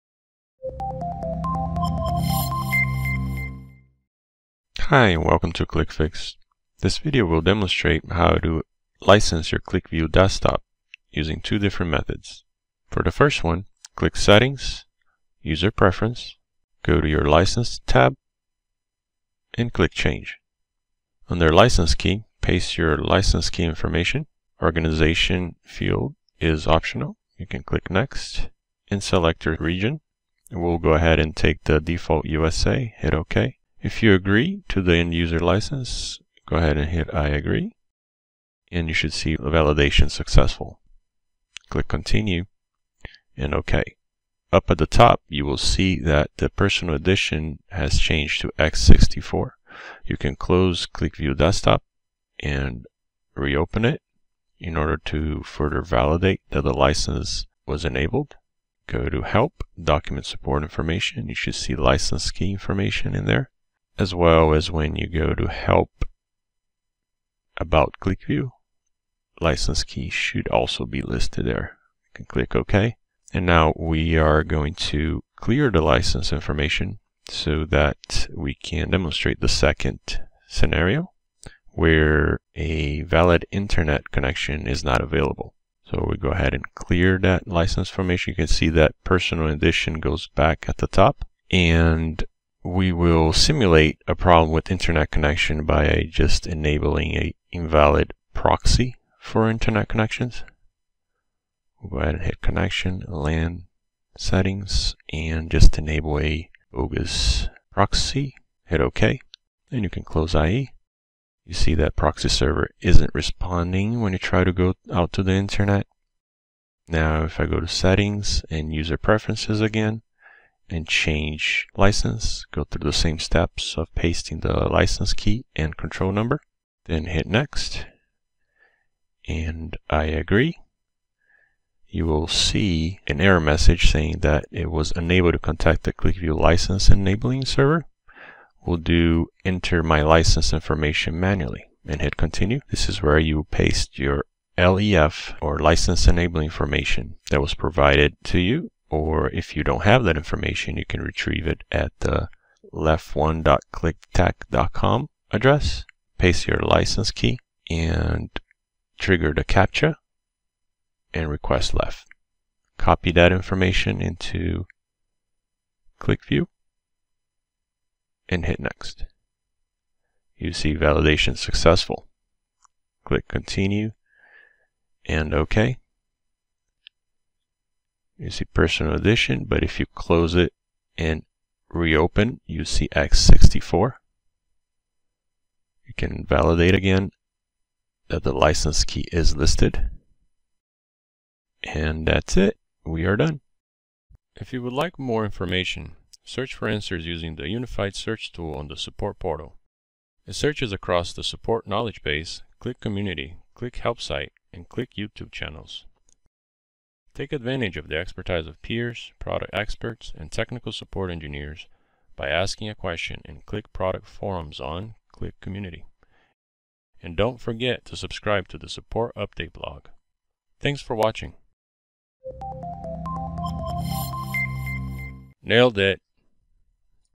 Qlik Fix: How to Activate QlikView Desktop with License Key or LEF
This video demonstrates how to activate QlikView Desktop using a license key & control number or using a LEF (License Enabler File) if there is no internet connectivity.